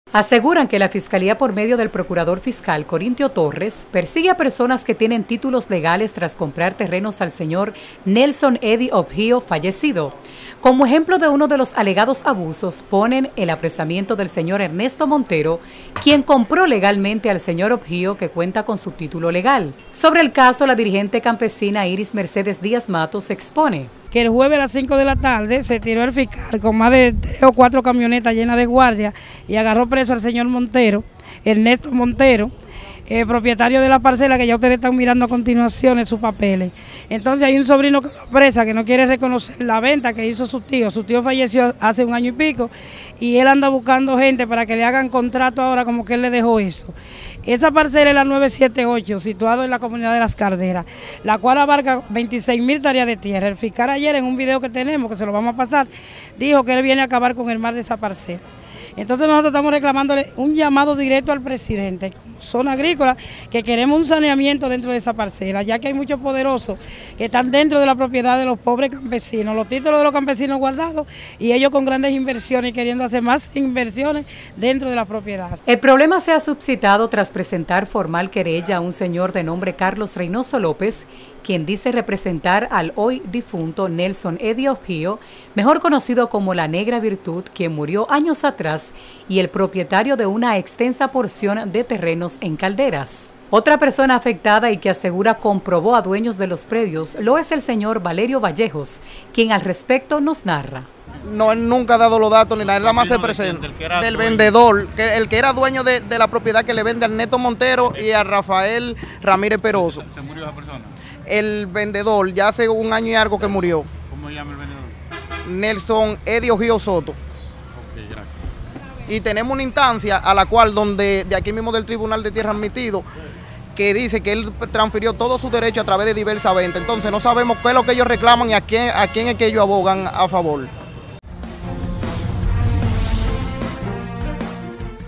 En Calderas denuncian atropello contra dueños de predios
Moradores de la zona de Calderas y dirigentes comunitarios del área, denuncian la puesta en práctica de atropello contra personas que compraron legalmente predios.